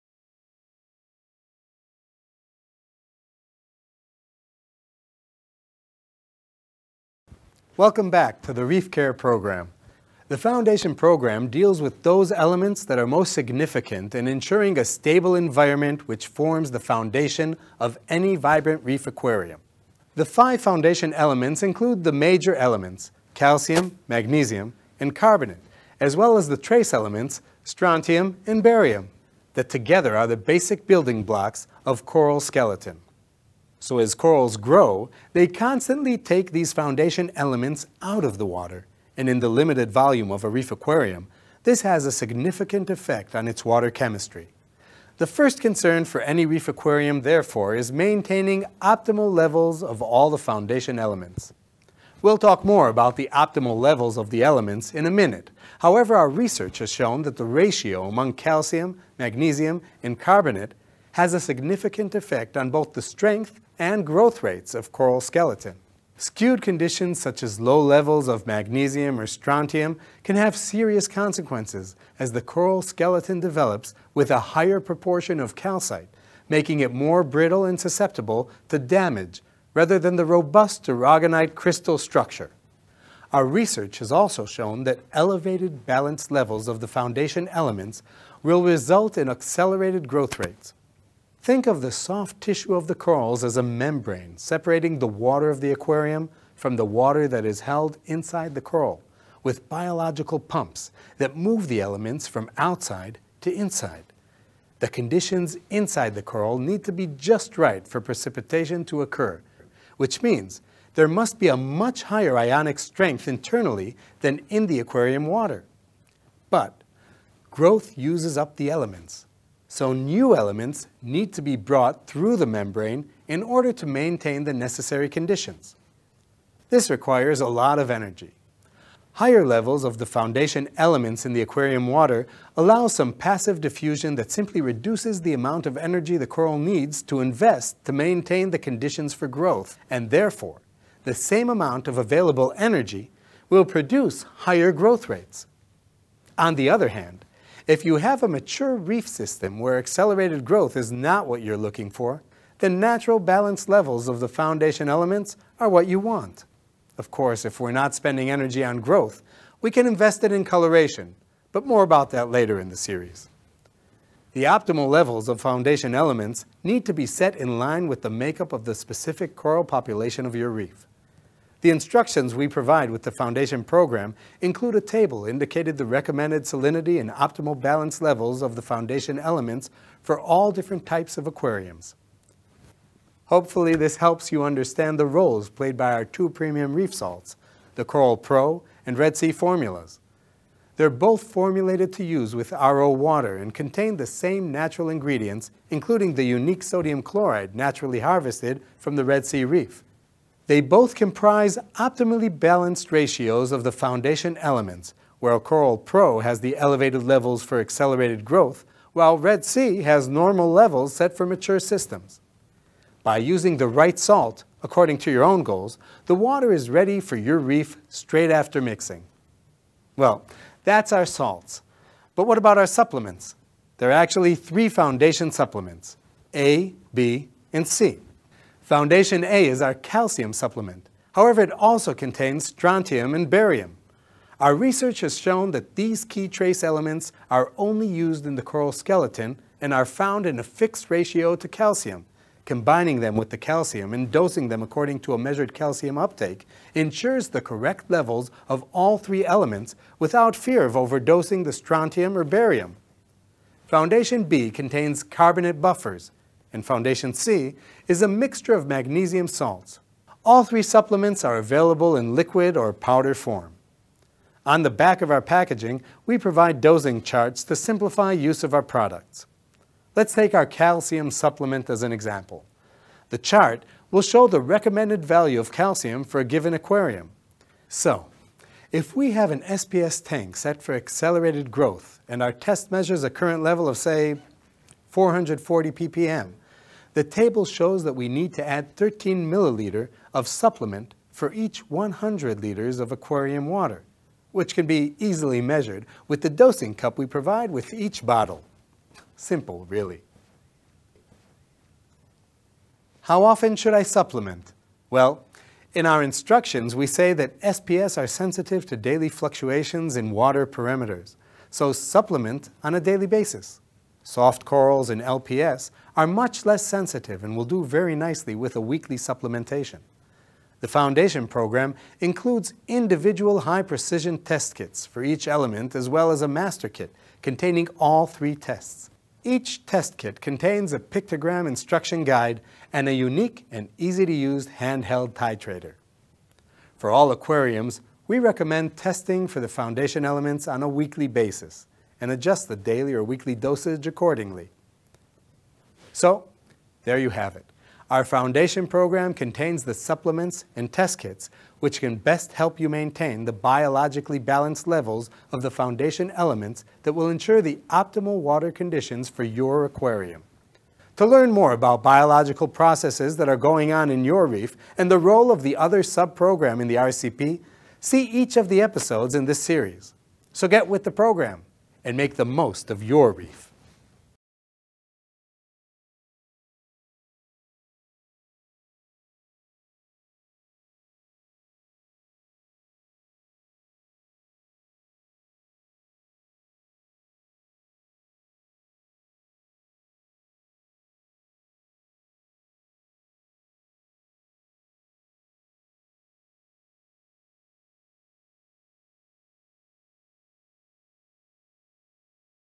Red Sea Reef Care – El Programa Reef Foundation (episode 2/5)
El Programa Reef Foundation de Red Sea está basado en nuestros años de experiencia en el desarrollo de soluciones para el mantenimiento sostenible de los arrecifes. Para proporcionar unos parámetros óptimos en el agua para el desarrollo del arrecife, se deben mantener unos niveles óptimos de los elementos básicos fundamentales: Calcio, Magnesio y Bicarbonatos. El Programa Reef Foundation proporciona suplementos y kits de prueba para lograr una química del agua óptima y los procesos biológicos de coral, lo que ayuda a obtener los resultados que usted necesita.

Aprender más acerca de:
Programa de Cuidado ” Reef Foundation” Red Sea: